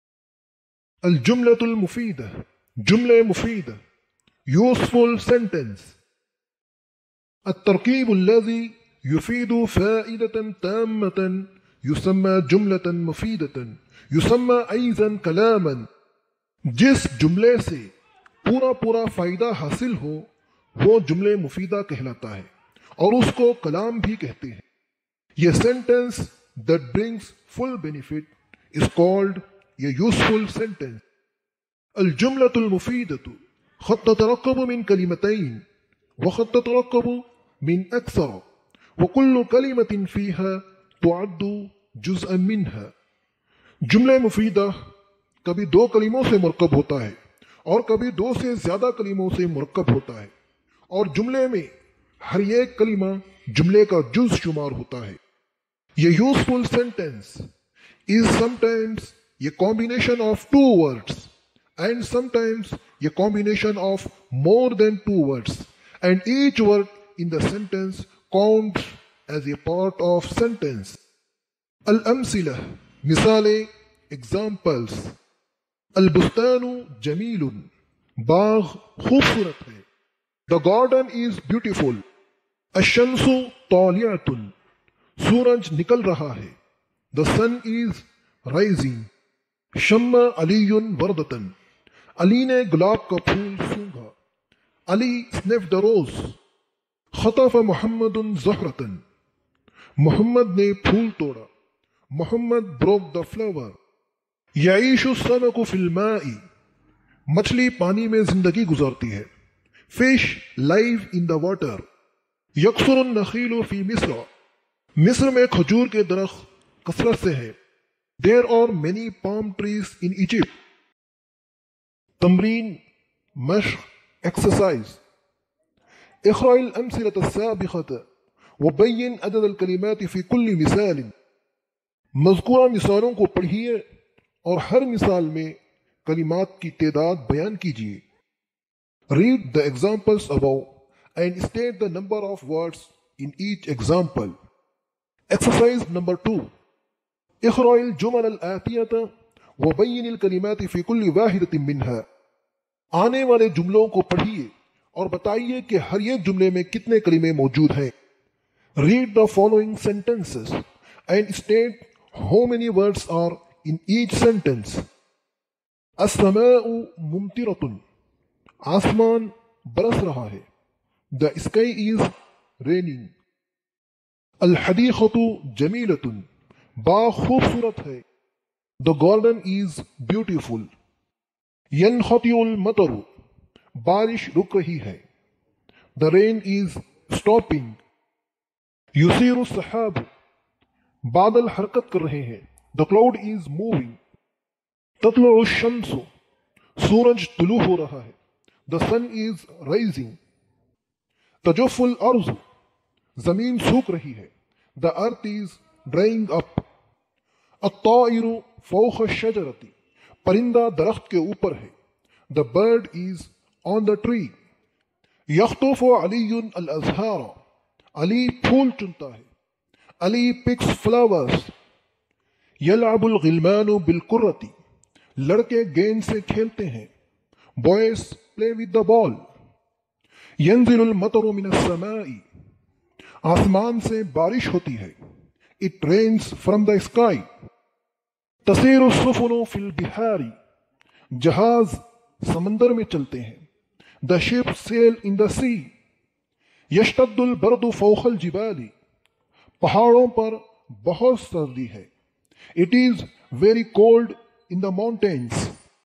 الجملة المفيدة جملے مفیدہ useful sentence
النحو الواضح الجلد الأول
الدرس الثاني أجزاء الجملة

annahwul wadeh aljildul  awwal
addarsus sani ajzaul jumla